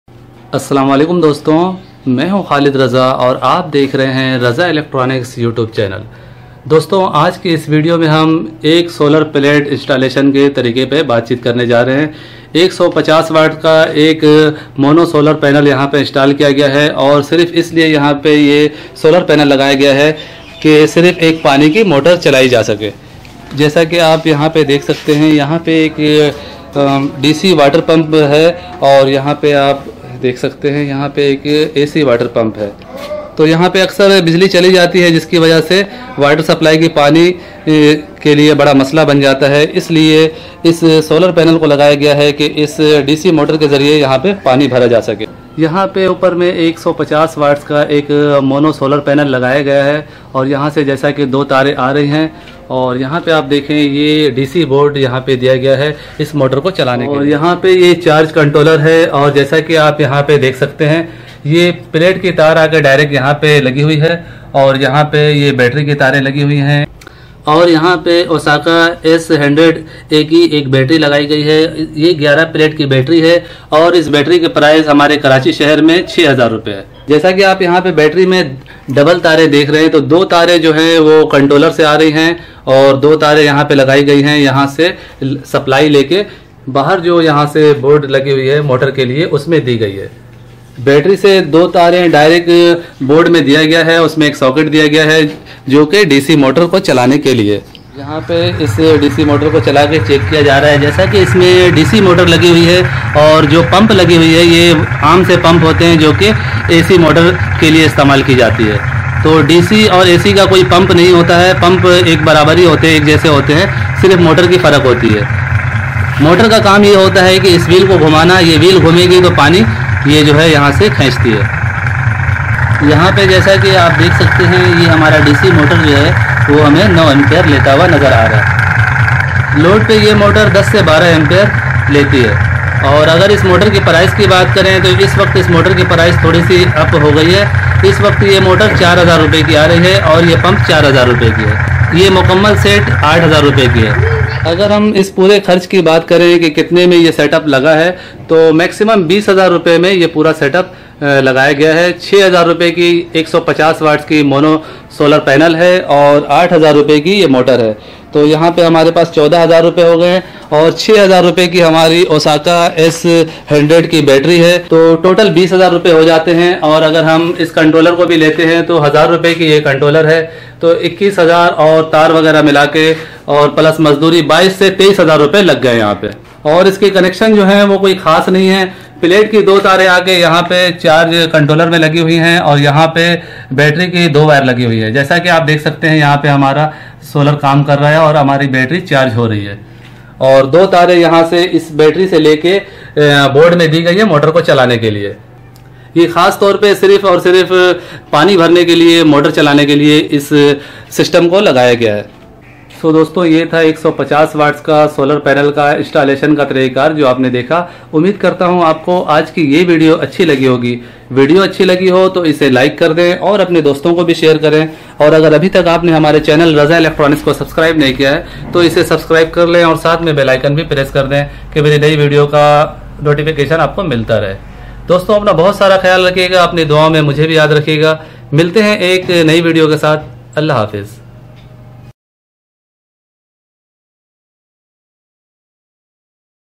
DC Water Pump And 175 Watts Solar Penal Installation In Urdu Hindi
Is video me 175 watts solar penal se dc water pump challane ka tariqa tafseel batay gaya he
i hope ke is video ko dekhne se ap ke knowledge me izafa hoga video achhi lage to isse like aur shear zaroor kare aur mere channel raza electronics ko subscribe karke bell button press karde